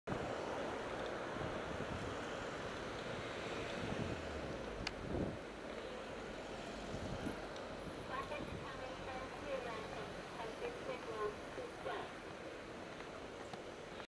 Red Light Jumper
Need I say anymore! All it took was 30 seconds for the lights to change!